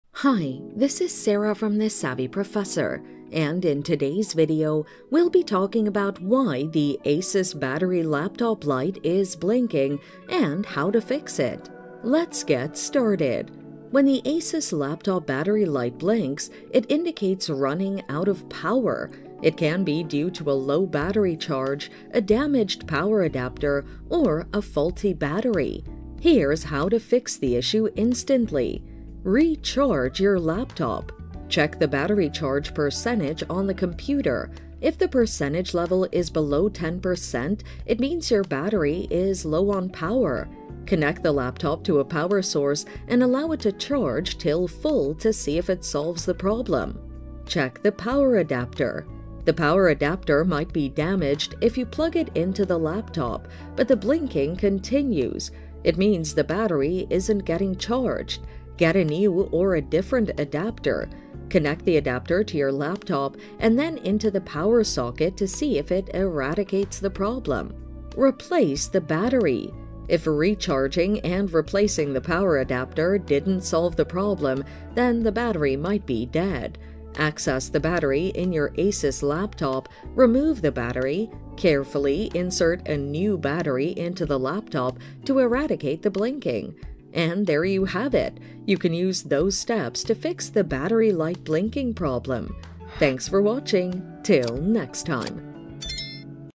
How To Fix Asus Battery Light Blinking (Out Of Power - What Are The Problems? - Best Guide!)
How To Fix Asus Battery Light Blinking (Out Of Power - What Are The Problems? - Best Guide!). In this video tutorial I will show you how to fix Asus battery light blinking.

When the Asus laptop battery light blinks, it indicates running out of power. It can be due to a low battery charge, a damaged power adapter, or a faulty battery.
Here is how to fix the issue instantly;
Recharge your laptop
• Check the battery charge percentage on your computer,
• If the percentage level is below 10%, it means your battery is low on power,
• Connect the laptop to a power source and allow it to charge till full to see if it solves the problem.
Check the power adapter.
• The power adapter might be damaged if you plug it into the laptop, but the blinking continues. It means the battery is not getting charged.
• Get a new or a different adapter,
• Connect the adapter to your laptop and then into the power socket to see if it eradicates the problem,
Replace the battery
• If recharging and replacing the power adapter didn’t solve your problem, then the battery might be dead,
• Access the battery in your Asus laptop,
• Remove the battery,
• Carefully insert a new battery into the laptop to eradicate the blinking.